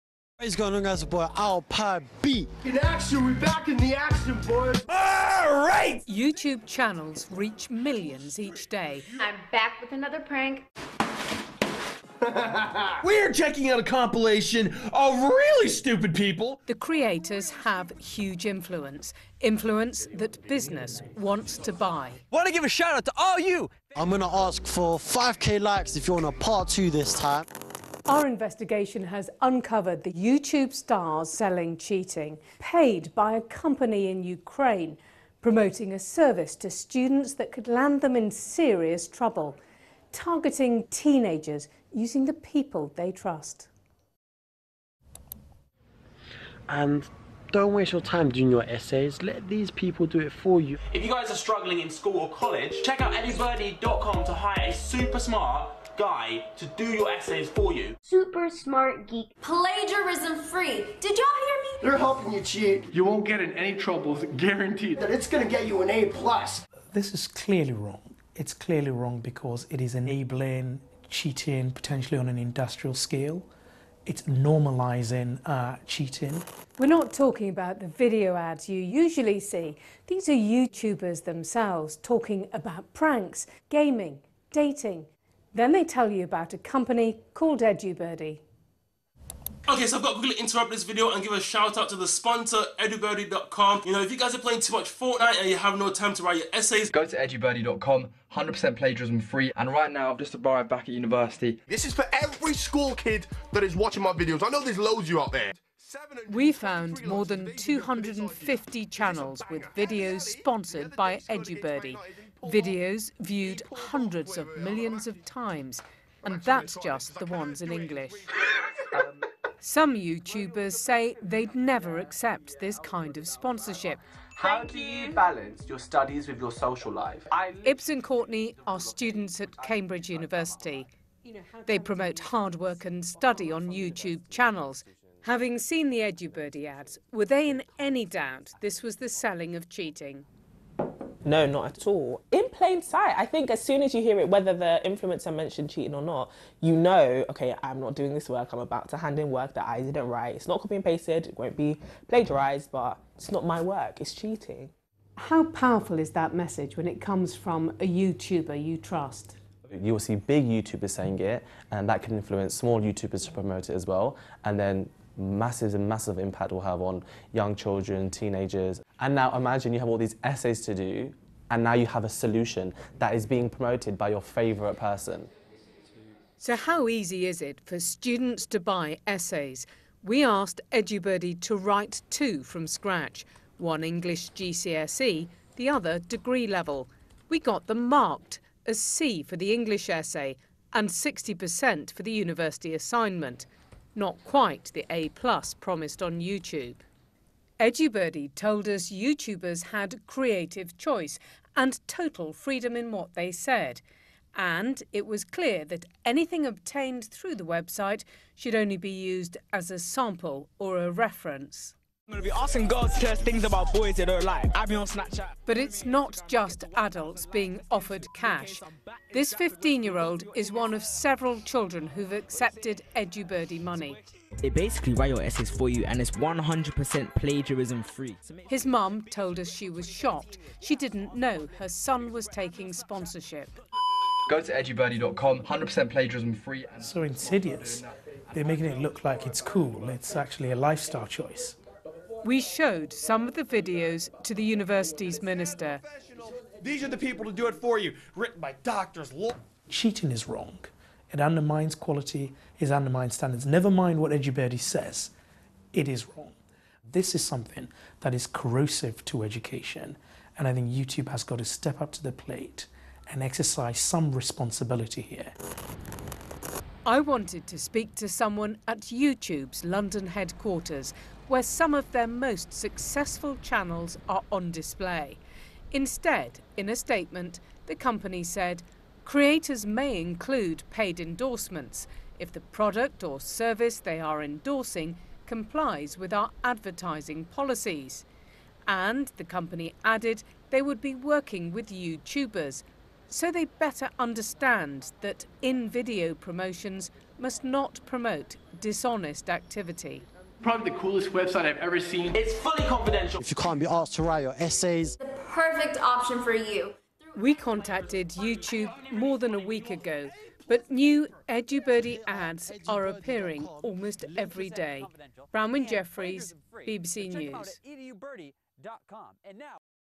EduBirdie: YouTube stars who are paid to sell cheating - BBC Stories
A BBC investigation finds that stars some YouTube stars are encouraging students to buy essays via EduBirdie. More than 250 channels are promoting EduBirdie, based in Ukraine, which allows students to buy essays, rather than doing the work themselves.
YouTube said it would help creators understand they cannot promote dishonest behaviour.